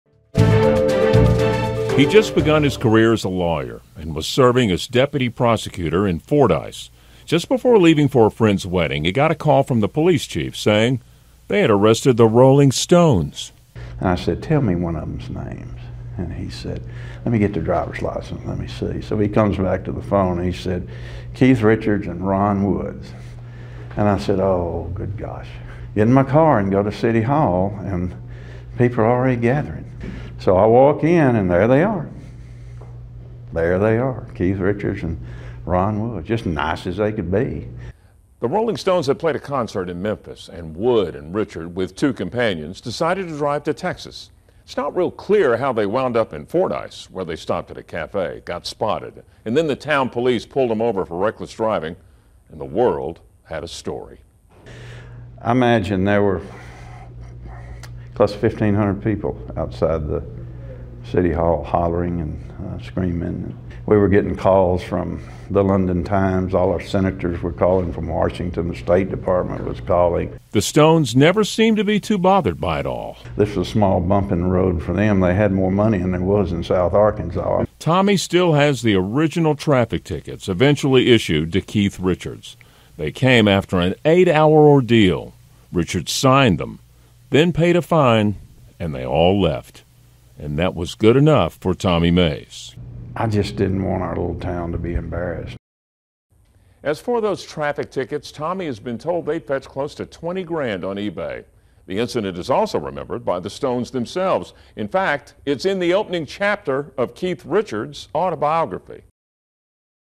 Prosecutor recalls Rolling Stones arrest 40 years later
Hall of Fame rockers Keith Richards and Ron Wood were arrested in Fordyce, Arkansas for reckless driving 40 years ago. One deputy prosecutor recalls the arrest and the significance it still holds for he town.